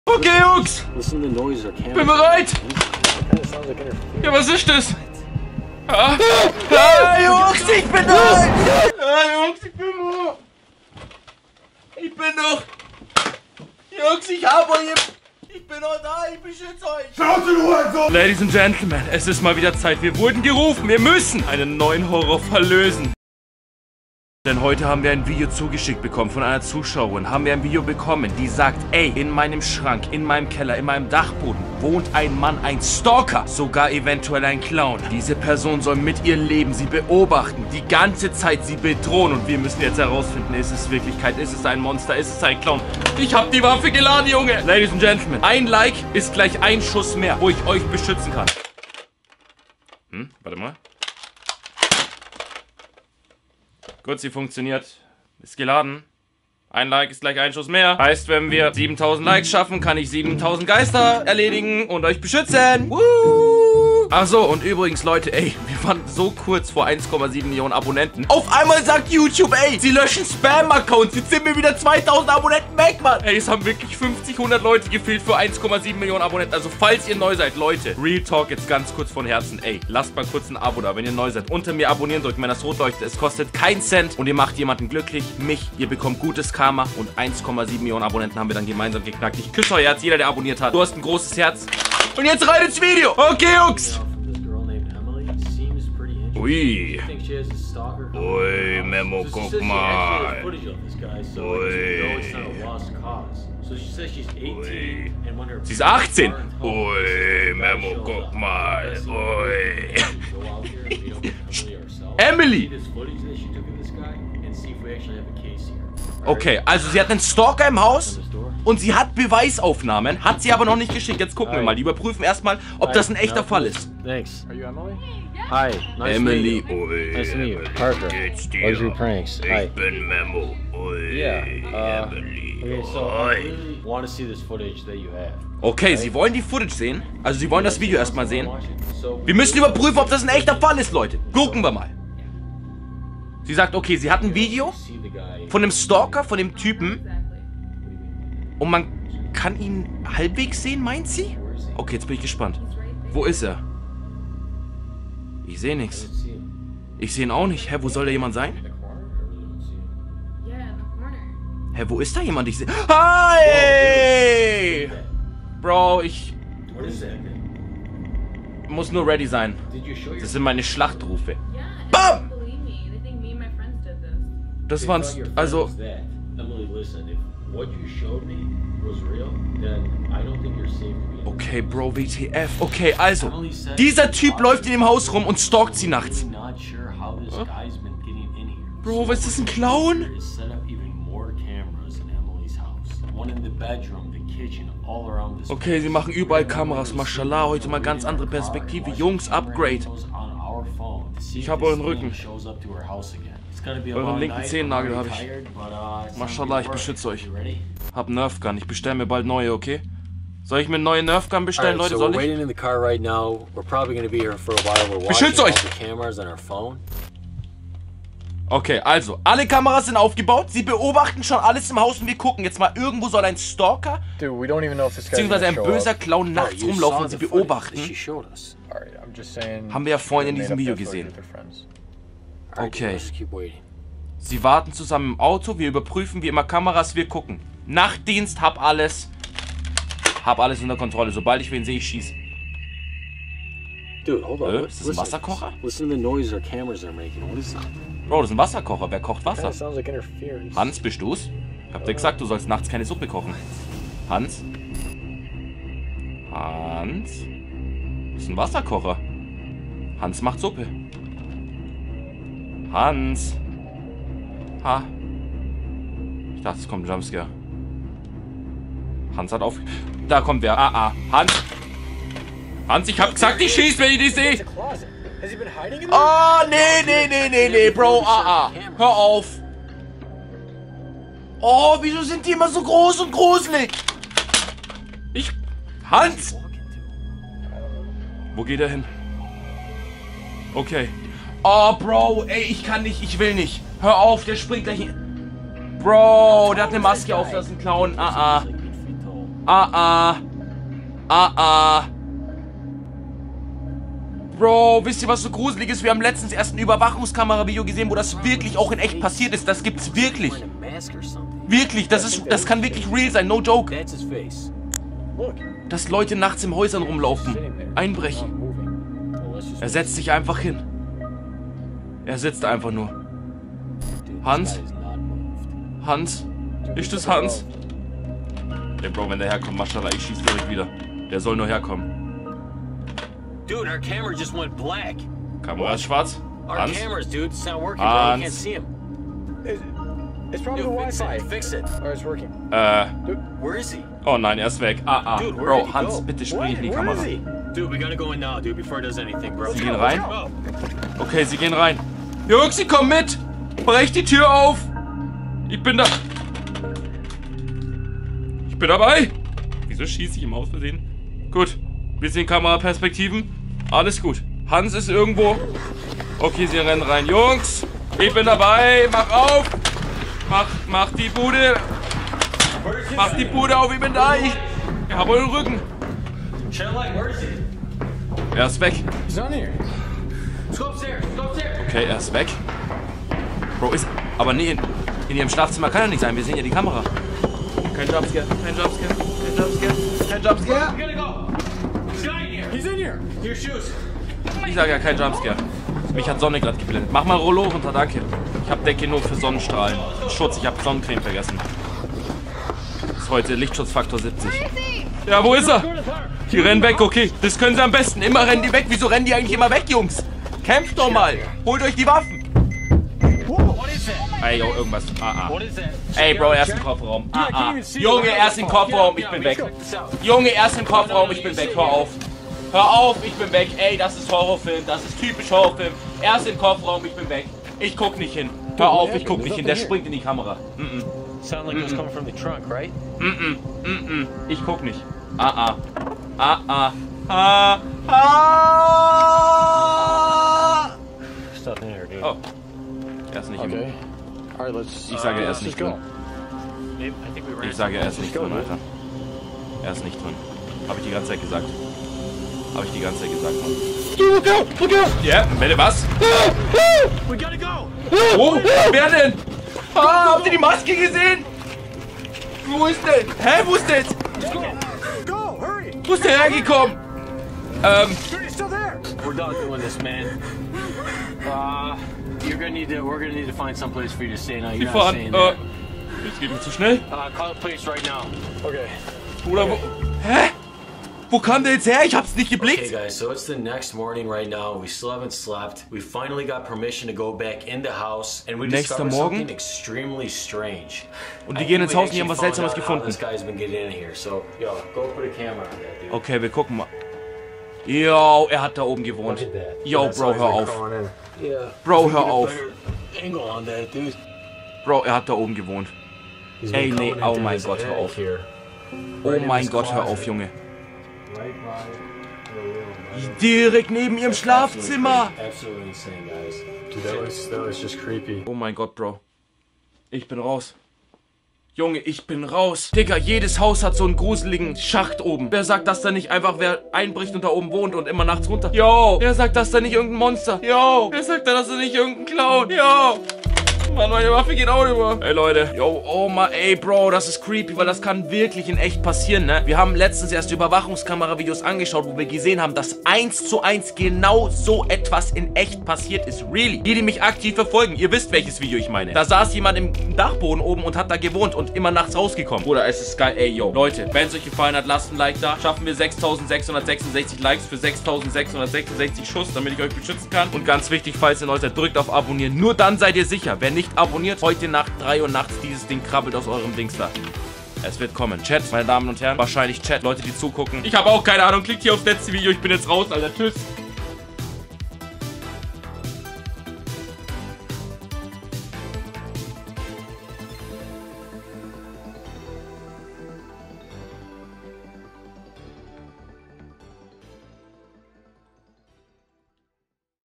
Wir Haben Einen STALKER In Ihrem KELLER GEFUNDEN.. (hilfe)
Wir haben einen Stalker in ihrem Keller gefunden hilfe! Gruselige Horror Videos um 3 uhr nachts wie rebekah wing reaktion deutsch!

► Video liken

FERTIG!